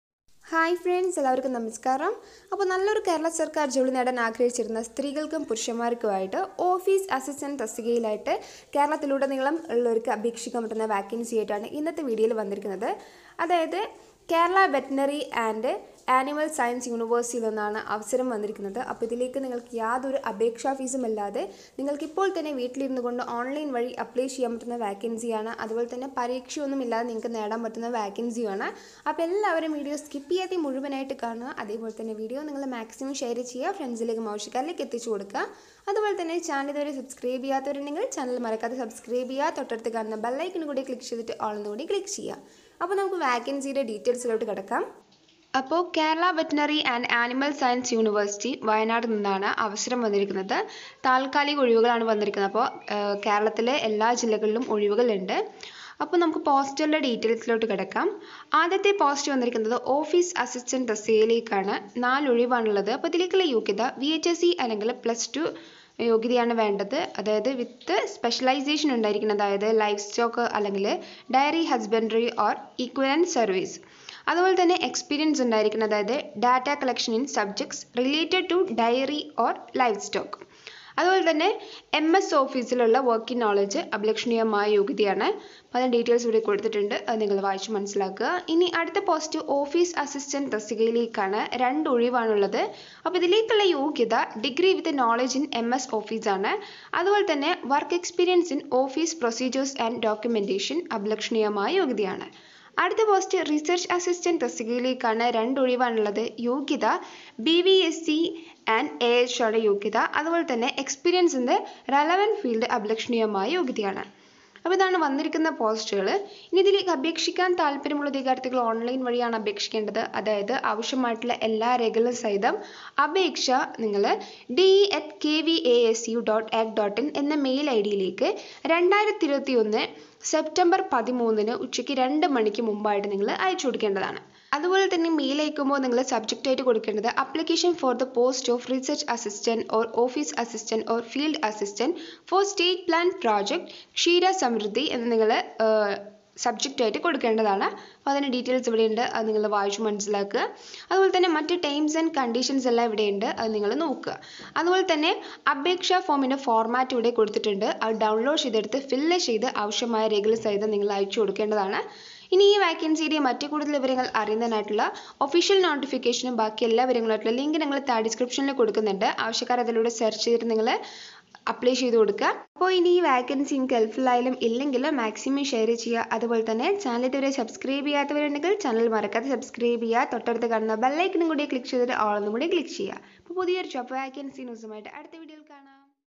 govtjob GovtJobs2020 Keralasarkarjob jobvacancymalayalam
നല്ല ശമ്പളത്തിൽ സർക്കാർ ജോലി നേടാൻ ആഗ്രഹിക്കുന്നവര്‍ക്ക് കേരള വെറ്റിനറി ഇന്സ്ടിട്യൂട്ടിൽ് അസിസ്റ്റന്റ് ആവാം